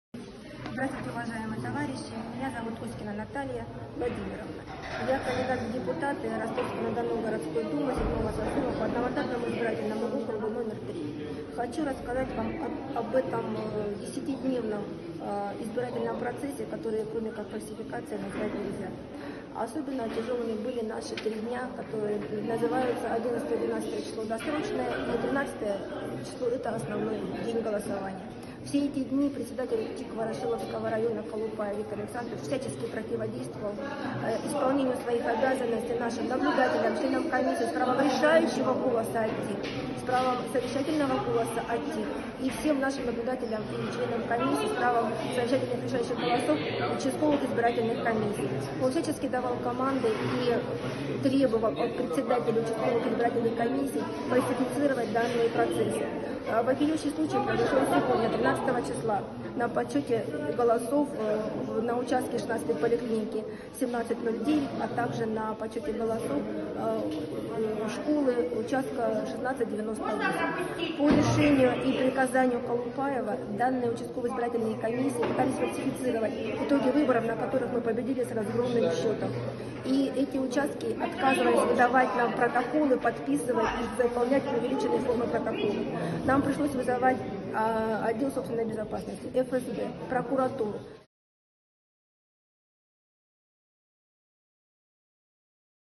Ростов-на-Дону: кандидат в депутаты Наталья Оськина рассказывает о нарушениях на участках
11–13 сентября 2020 года проходят выборы губернатора Ростовской области. Регионом последние 10 лет руководил Василий Голубев. На эти выборы он тоже выдвинулся. Здесь мы собрали все, что известно о кандидатах на должность главы региона.

161.RU рассказывает в режиме онлайн о том, что происходит на участках в основной и последний день голосования.